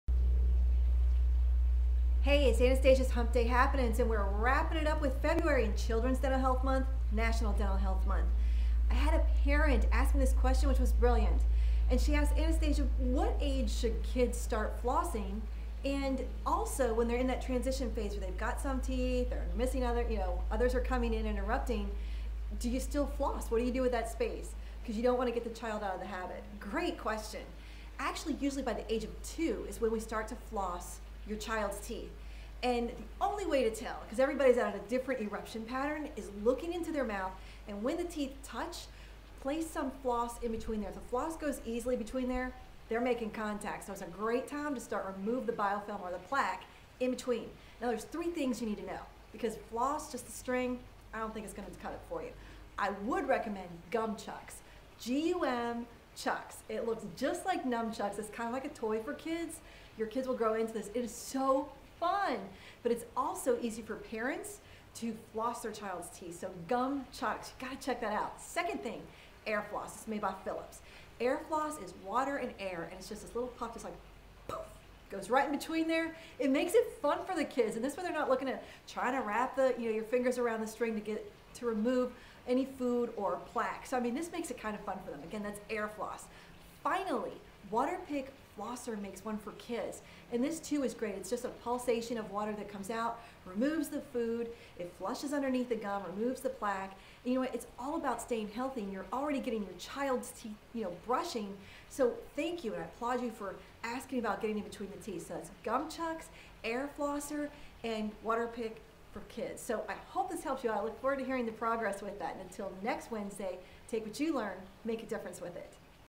Anastasia's Hump Day Happenings: 3 Products to "Floss" your kids teeth!
Ever wonder what age you should start flossing your child's teeth? Or what to do when they have gaps between them while losing baby teeth?